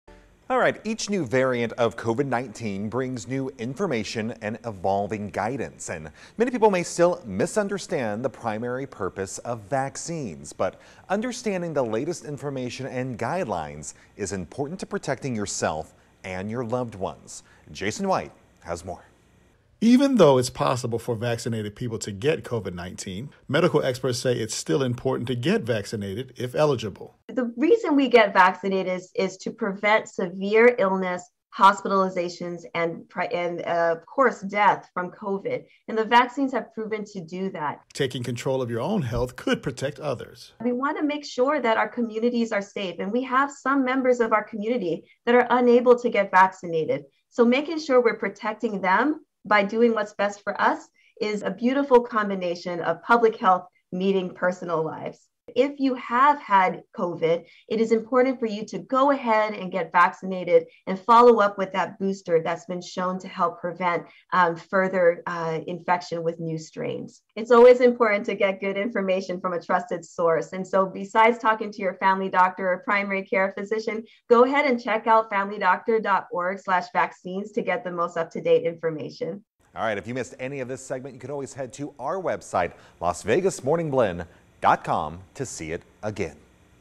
COVID-19 Variant Breaks Records
Family physician Dr. Marie-Elizabeth Ramas answers questions about vaccines, boosters, testing, and immunity